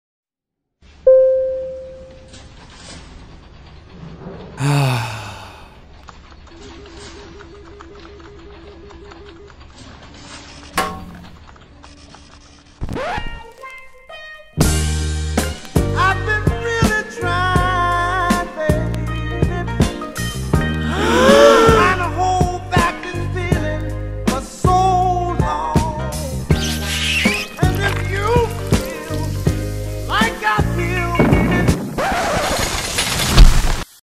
Amended Animation "Fizzed Up"
Fizzed up a rom com gone wrong. When vending machines fall in love. First year final project. Team consisted of Matthew Duddy, Glenn O Neil, Lauren Bell and David Pollock